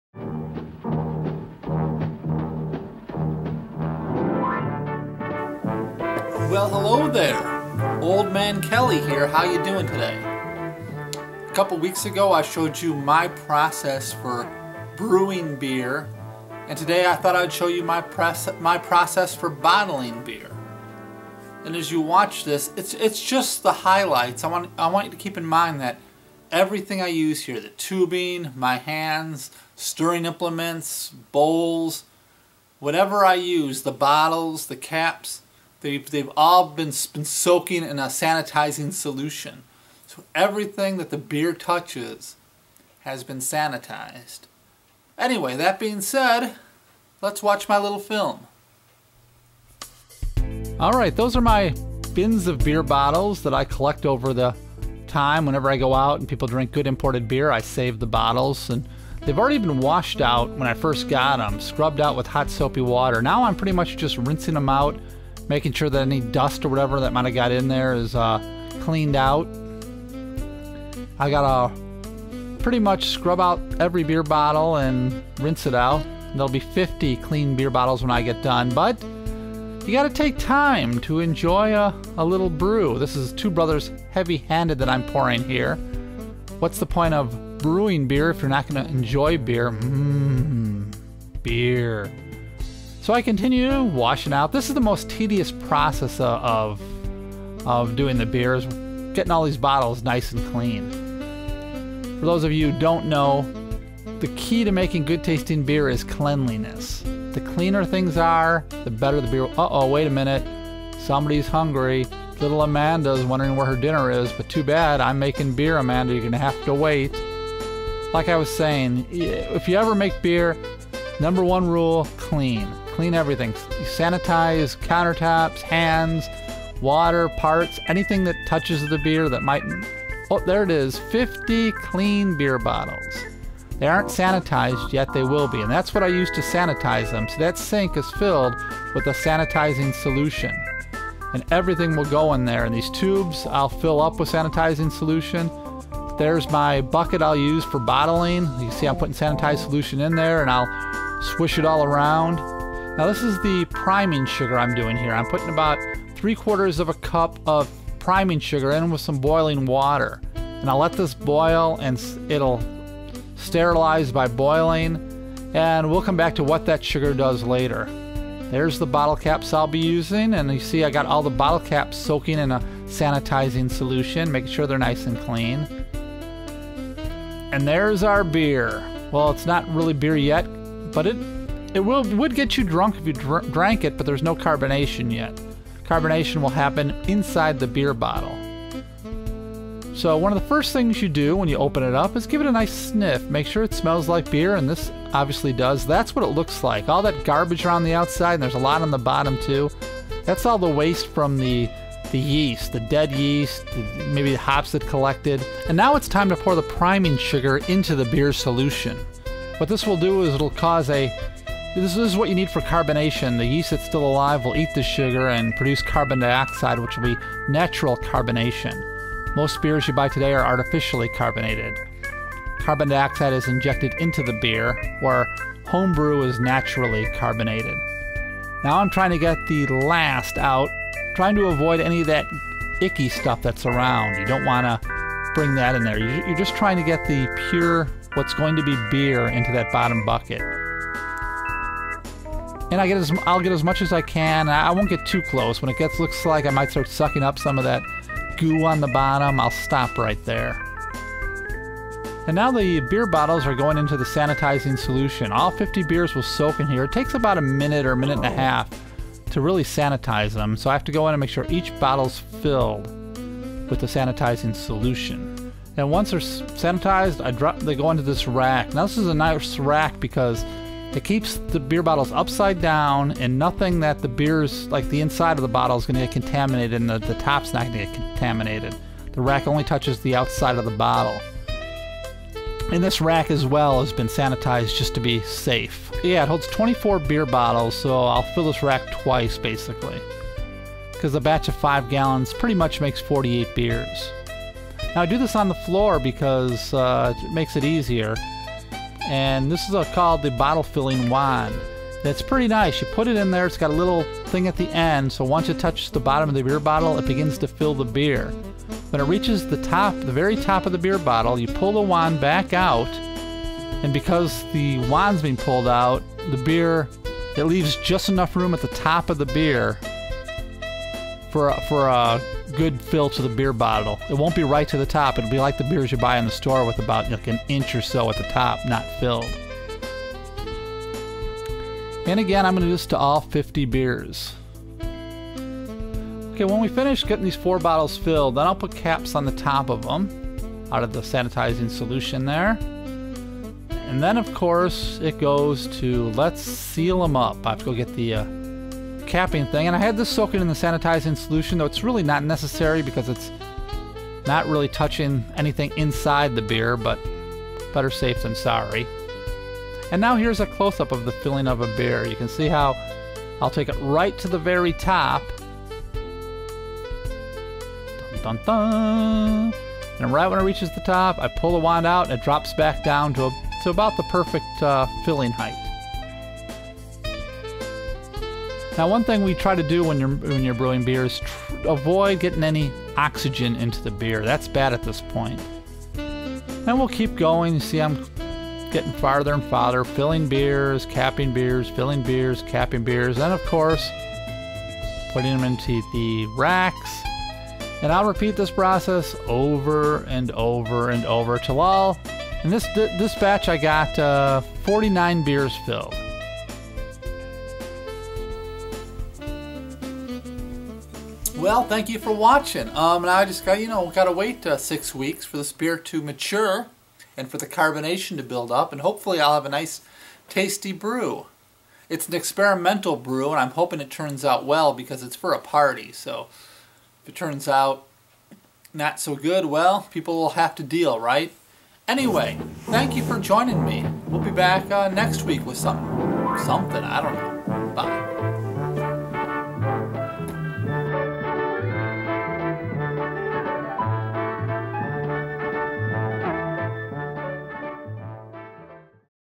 The Old Man Bottles Beer
Two weeks ago I brew 5 gallons of beer. Today I put that brew into bottles. Join my, and feel free to tell me what I am doing